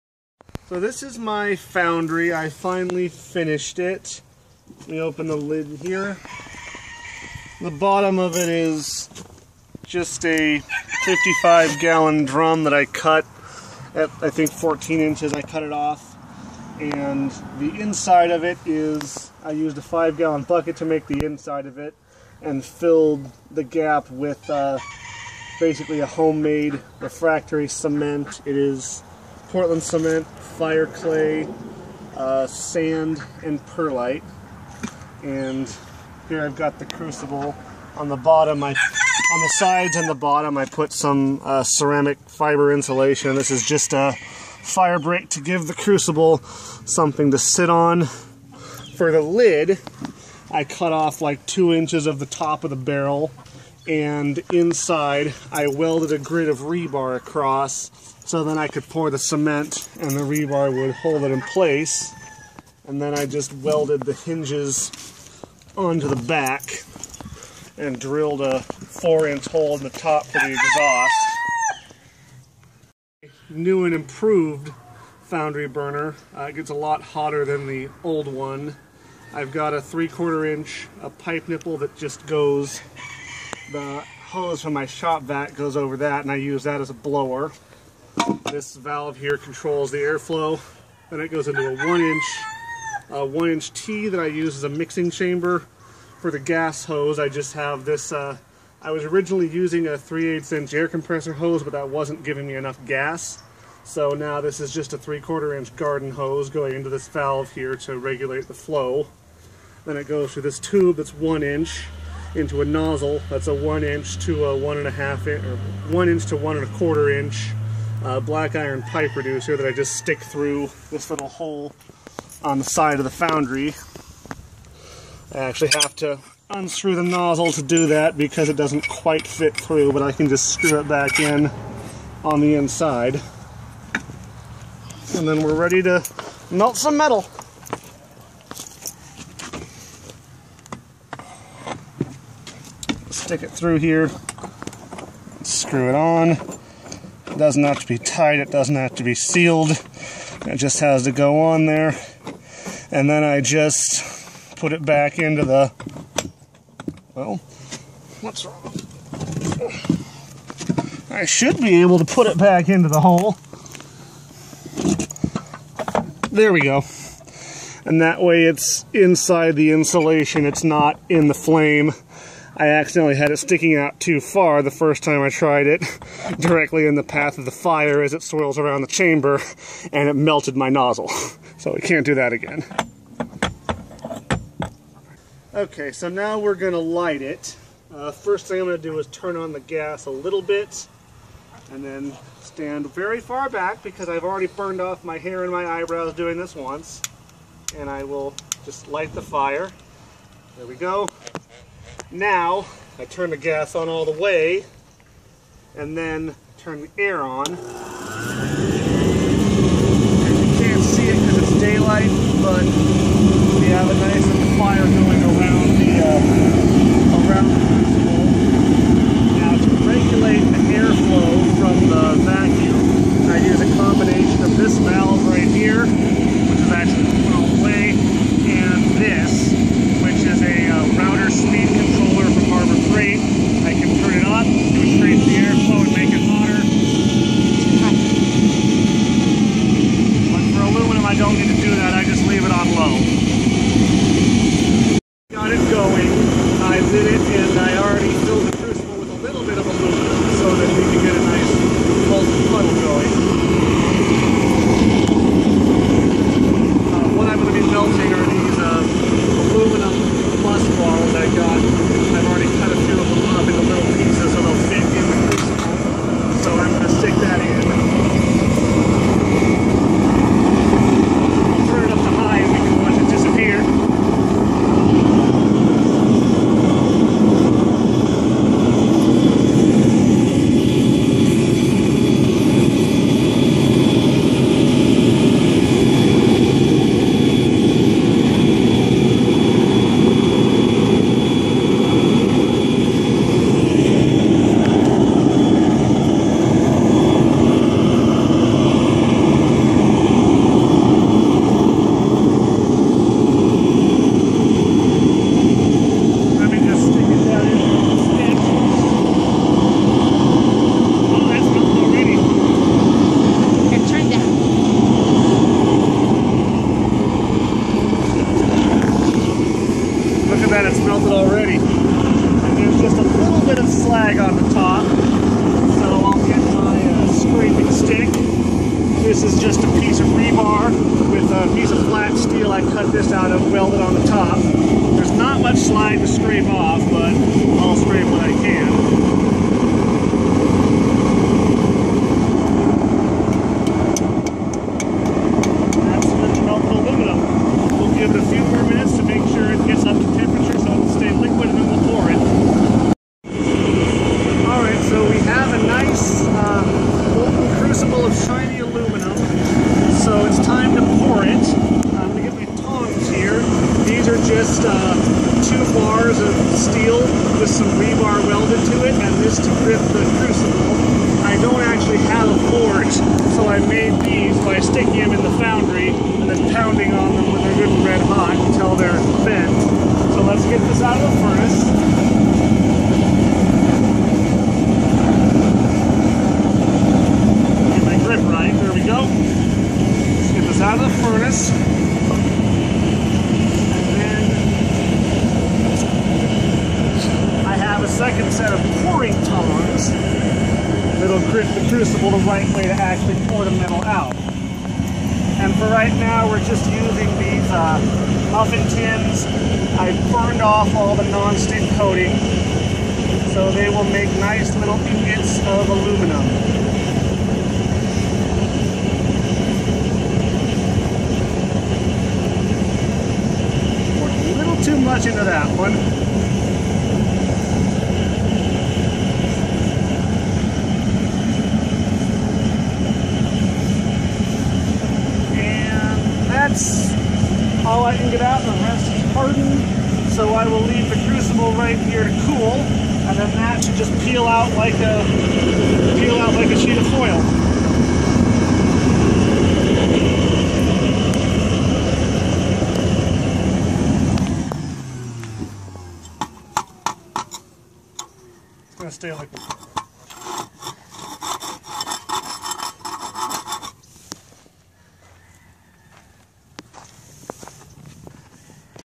Melting aluminum in the home foundry
First try melting aluminum in our foundry